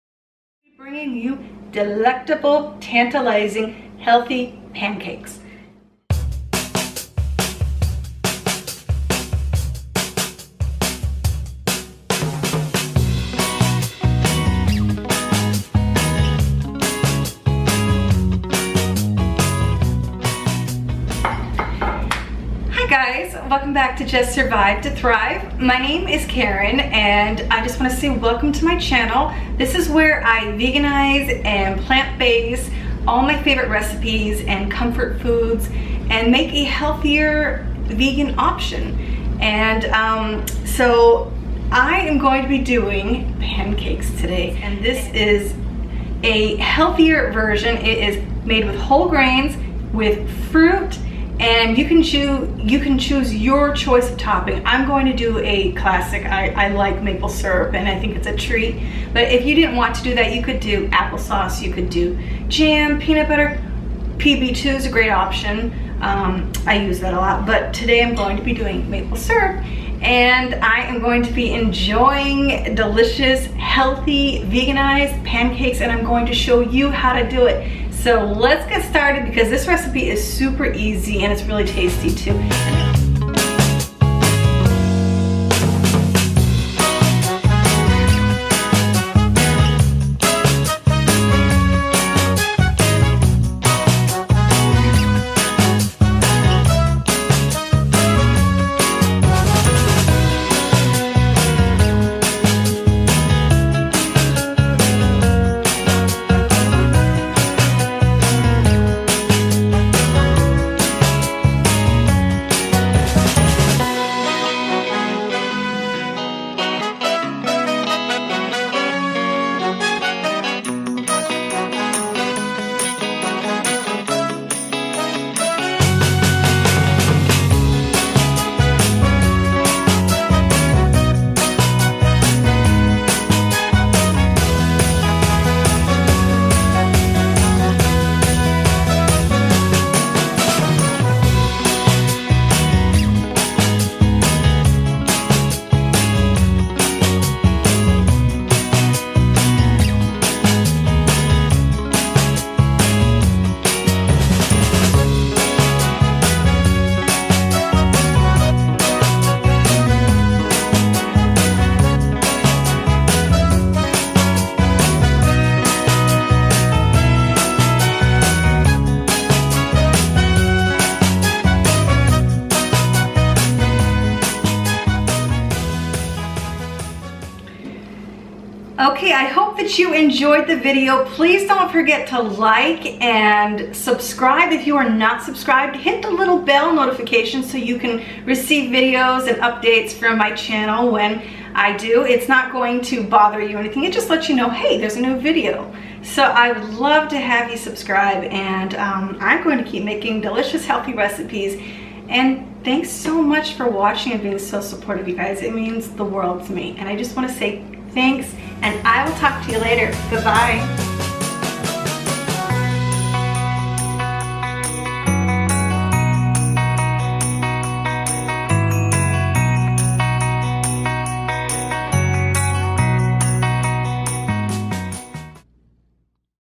EASY HEALTHY VEGAN PANCAKES | Whole Grains & Fruits
Easy and healthy vegan pancakes that are made with fruit and whole grains. You can still enjoy this favorite comfort food without any guilt.  Who doesn't love a delicious stack of pancakes?! This pancake recipe allows you to enjoy a healthy breakfast and eat your pancakes too.

PANCAKE RECIPE:
1 1/2 cup oats, ground into flour
1 cup almond milk
1 ripe banana
1 Tbsp coconut sugar
2 tsp baking powder
1 Tbsp lemon juice
fresh fruit of choice for layering

1. Process oats into a flour in a blender and add the remaining ingredients. Blend until the batter is well combined.
2. Let the batter rest for 5 to 10 minutes to thicken.
3. Use a really good nonstick frying pan or you may need to use a bit of cooking spray. Heat pan on low to medium heat and cook pancakes until bubbles are forming and ends are turning golden. Try not to flip too soon, so the pancakes flip much easier.
4. Layer with fruit and top with your choice of sweetener.